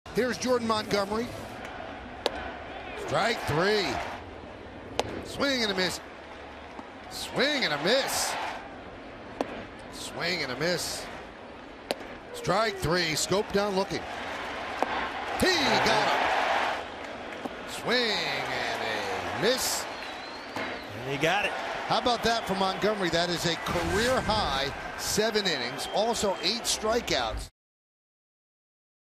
BAL@NYY: Montgomery fans eight over seven innings
Jordan Montgomery struck out a career-high eight batters over seven innings of two-run ball to lead the Yankees past the Orioles